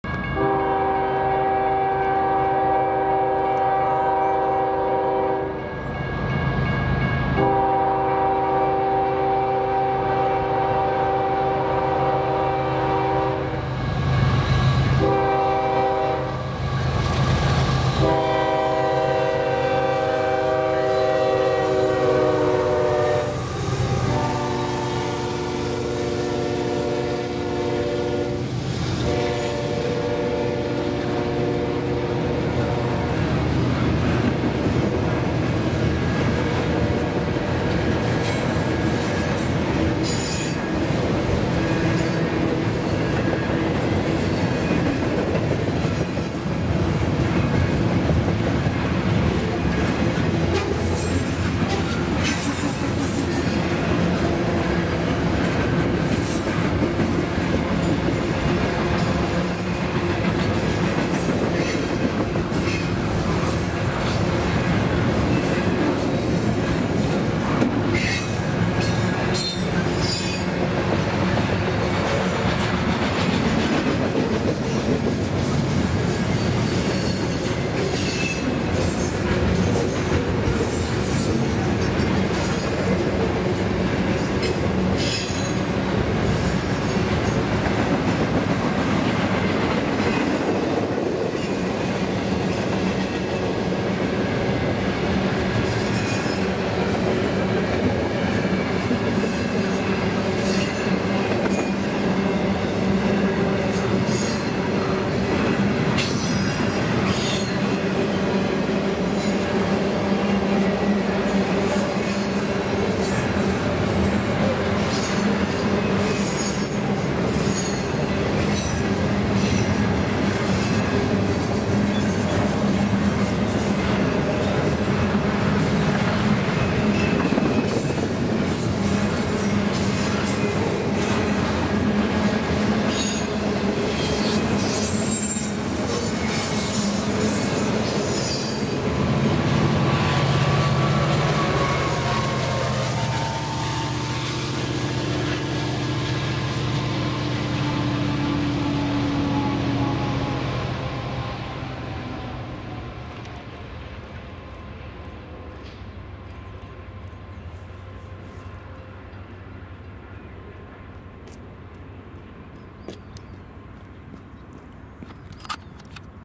part 2 UP mixed intermodal on the UP Golden State Route 10/13/2016 # 2
continuation of the WB I-DULB mixed at MP 298.4 UP Pratt Sub Pratt, Kansas